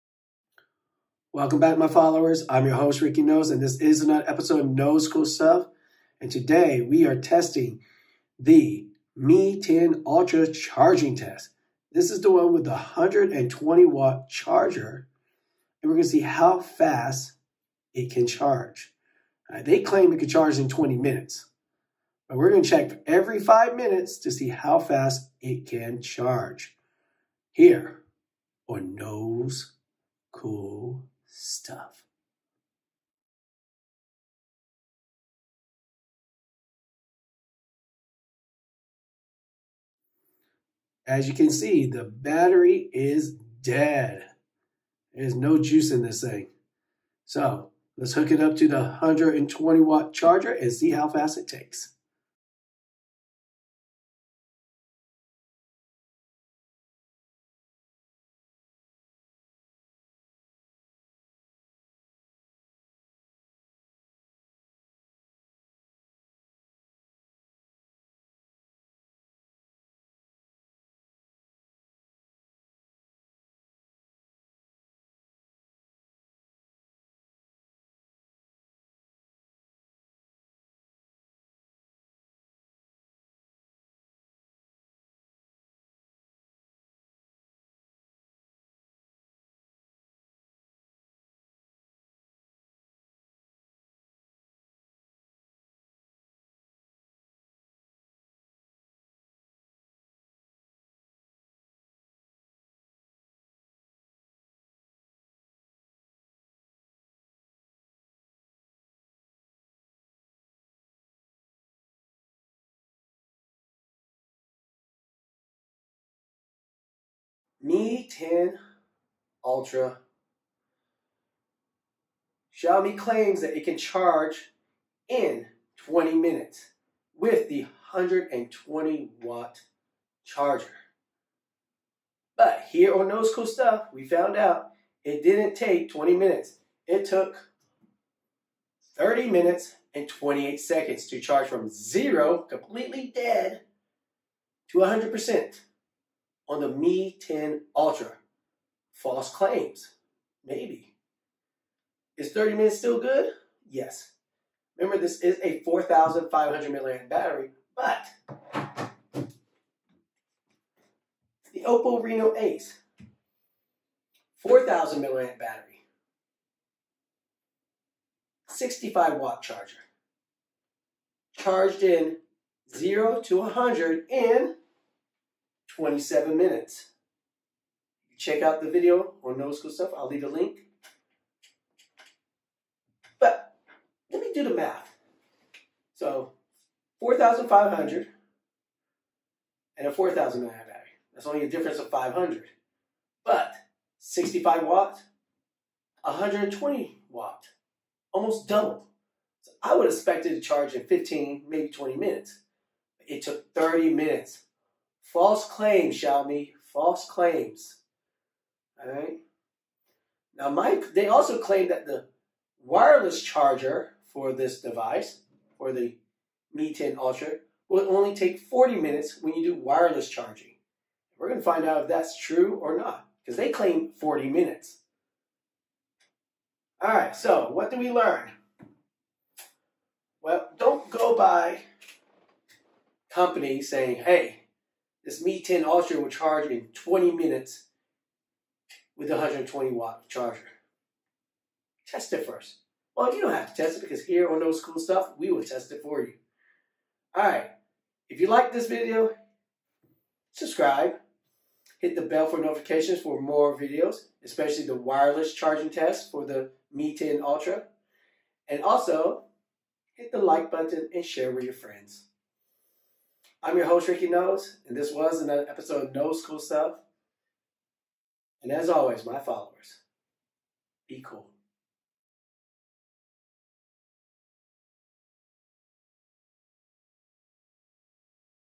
Mi 10 Ultra Charging Test: Xiaomi Claims 23mins to charge 0% to 100% - FALSE !!!
Mi 10 Ultra comes with a 120w charger and Xiaomi claims it can charge in 23 minutes. Here at Knoles CoolStuff we proved them wrong. Watch and see how long it took for the Mi 10 Ultra to charge from 0% to 100%,